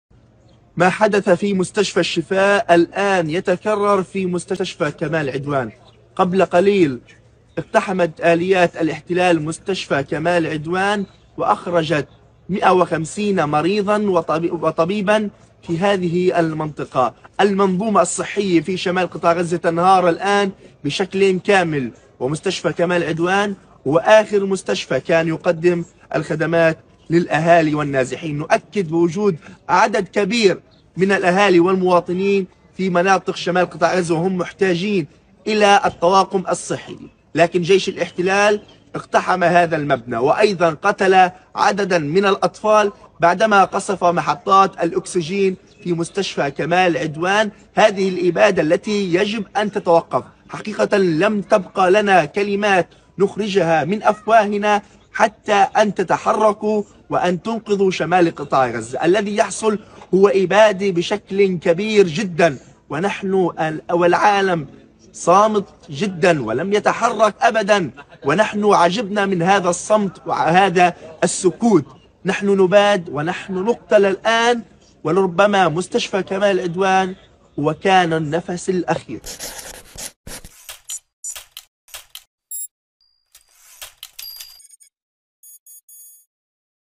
"ما حدث بالشفاء يتكرر بكمال عدوان"
"ما حدث في مستشفى الشفاء يتكرر الآن في مستشفى كمال عدوان.. الاحتلال أخرج 150 طبيبا ومريضا.. عجبنا من هذا الصمت والسكوت".. مستشفى كمال عدوان وحيدًا في مواجهة المـ ـوت